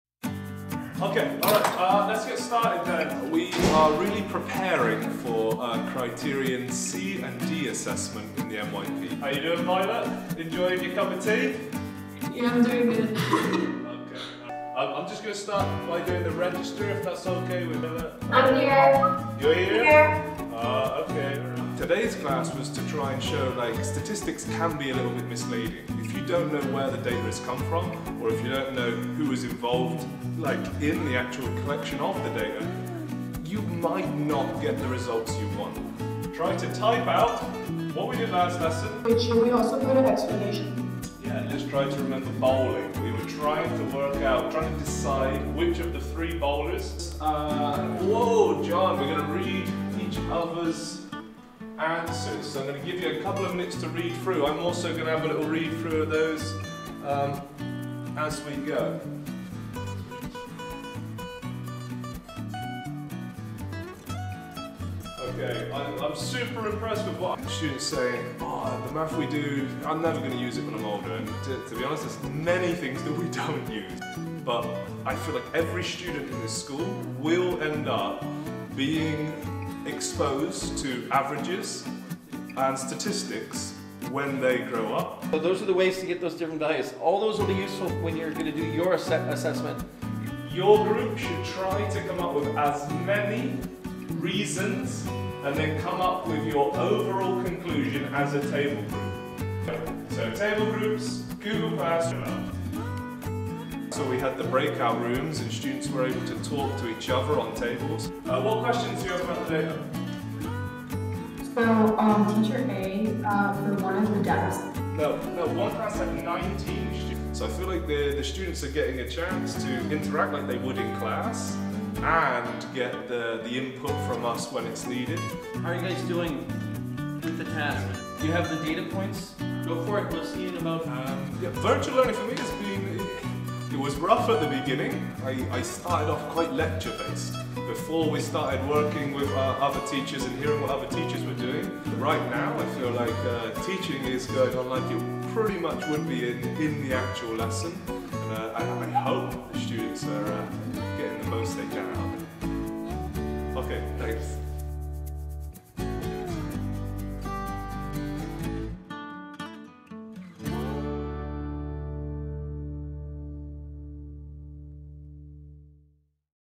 Standard Grade 10 Math, SFS High School Virtual learning
This video is a sample glimpse of what it looks like in one of our High School's virtual learning classroom. It has been a testing time for our existing educational institution due to the COVID-19 outbreak.
Through our continued effort in communicating, connecting and collaborating with parents, students and educators all around the world, the classroom atmosphere has changed very much to fit our current circumstances but our core of teaching has much stayed the same.

Featured in this video are Haydn Kneeshaw, BSc - IB Mathematics Teacher  and Adam Teather, Academic Lab Faculty in our High School who endeavors tirelessly to bring academic excellence to our students in quarantine.

Here we say a huge thank you to Mr Haydn Kneeshaw, Adam Teather and many other SFS faculty for their continued excellence and dedication in teaching.

Video by Juhie Suh @SFS Communication & Marketing

Filmed and edited by Juhie Suh at SFS Communications and Marketing.